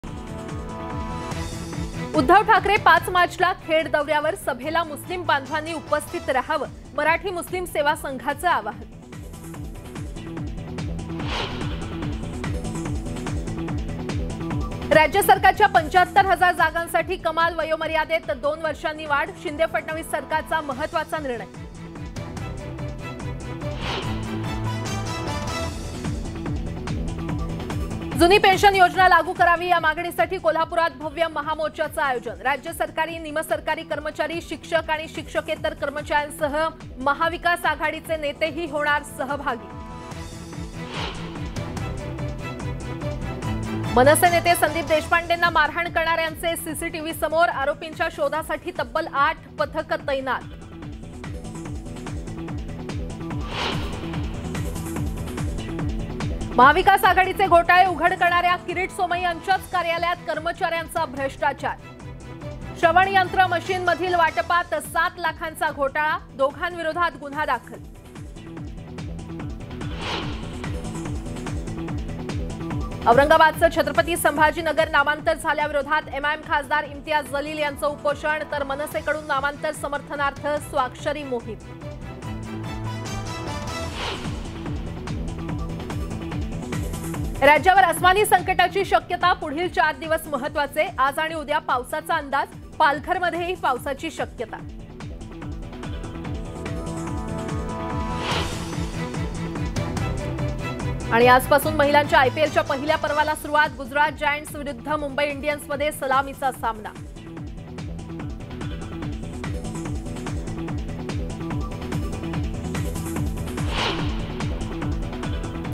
Marathi News Headlines | 9AM News Today | Latest Maharashtra News | News18 Lokmat | Mar 4 , 2023
There is a regular hearing in the Supreme Court on the power struggle in the state. Opposition to the renaming of Chhatrapati Sambhaji Nagar. Chain movement of 11 am in front of Kha Imtiaz Jalil collector office. Signature campaign of MNS in support of Chhatrapati Sambhaji Nagar renaming. A grand rally has been organized in Kolhapur to demand whether the old pension scheme should be implemented. In order to get old pension in this march, old pension rights organization along with state government semi-government employees, teachers and non-teaching staff will participate.

राज्यातल्या सत्तासंघर्षावर सर्वोच्च न्यायालयात नियमीत सुनावणी आहे. छत्रपती संभाजी नगर नामांतराला विरोध. खा इम्तियाज जलील जिल्हाधिकारी कार्यालयासमोर सकाळी 11 वा यांचे साखळी आंदोलन.छत्रपती संभाजी नगर नामांतराला समर्थन मनसे ची स्वाक्षरी मोहीम. जुनी पेन्शन योजना लागू करावी या मागणीसाठी कोल्हापुरात भव्य महामोर्चाच आयोजन करण्यात आलं आहे. या मोर्चात जुनी पेन्शन मिळावी यासाठी राज्य सरकारी निमसरकारी कर्मचारी ,शिक्षक व शिक्षकेतर कर्मचारी यांसह जुनी पेन्शन हक्क संघटना सहभागी होणार आहे.
n18oc_news